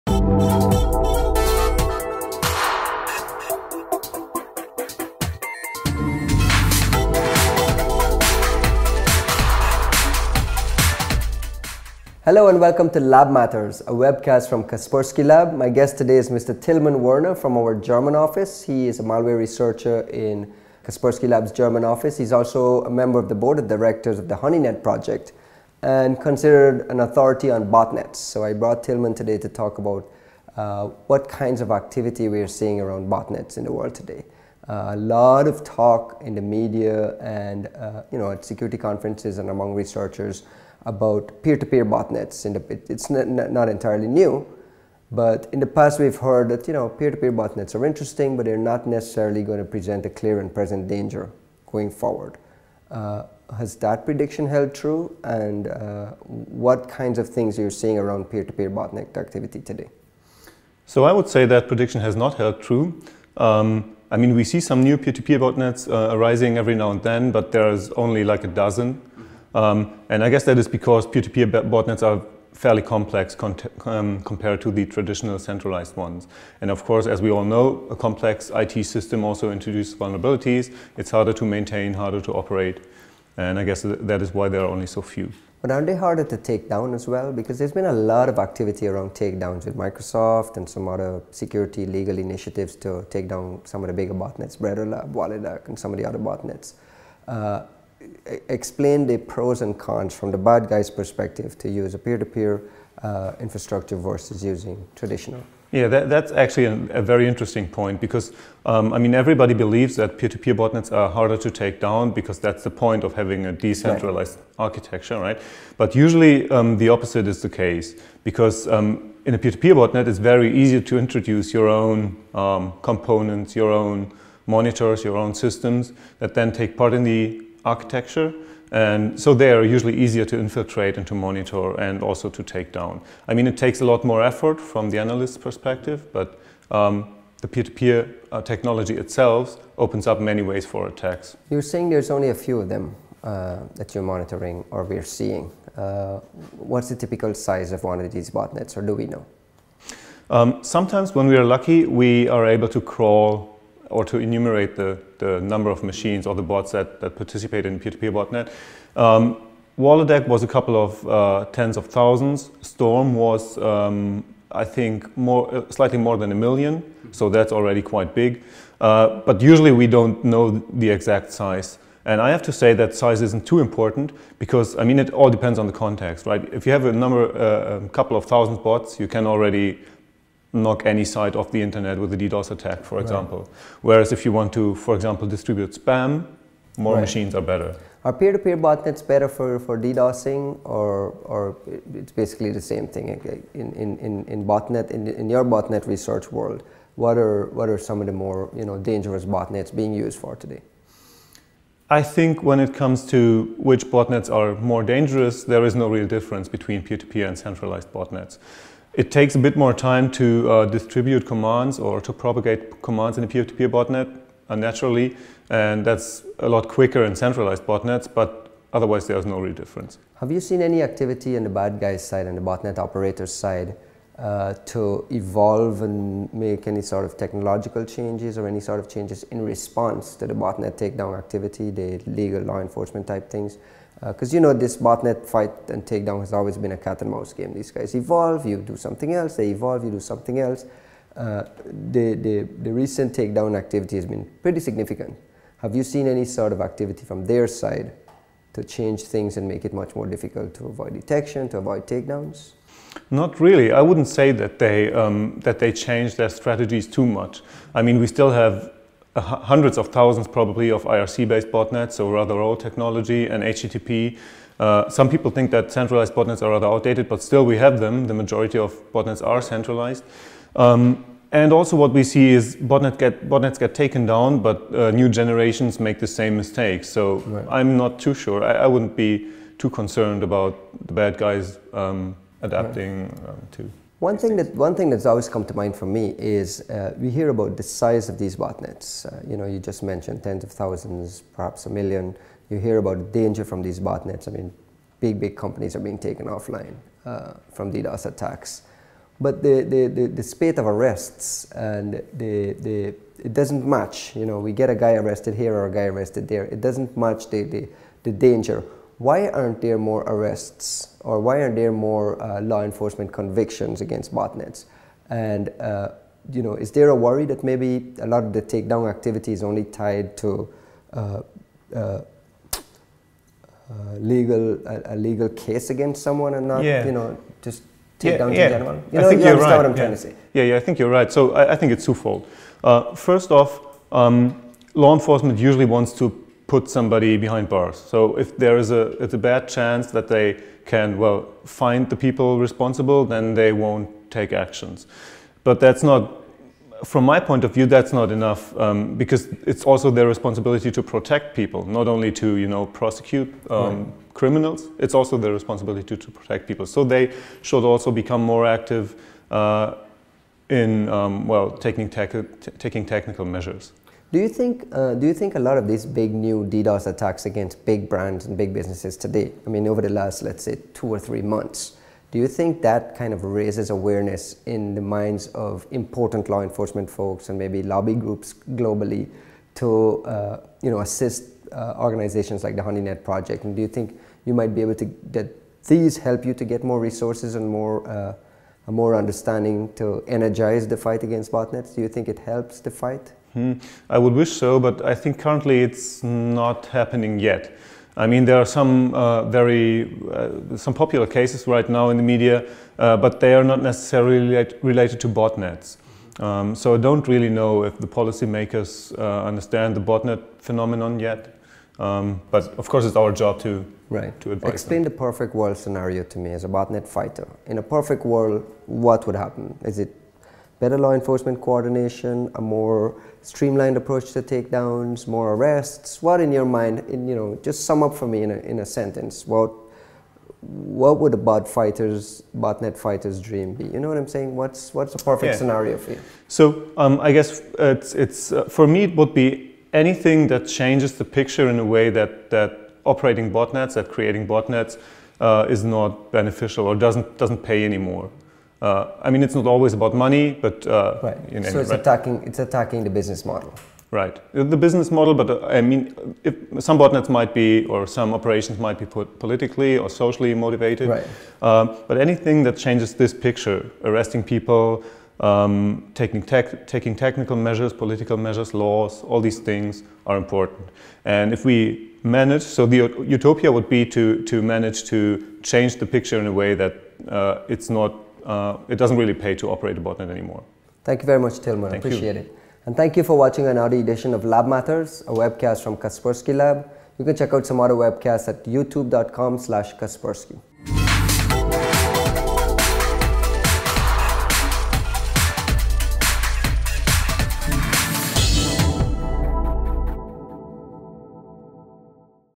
Lab Matters - The threat from P2P botnets
Kaspersky Lab malware researcher Tillmann Werner joins Ryan Naraine to talk about the threat from peer-to-peer botnets. The discussions range from botnet-takedown activities and the ongoing cat-and-mouse games to cope with the botnet menace.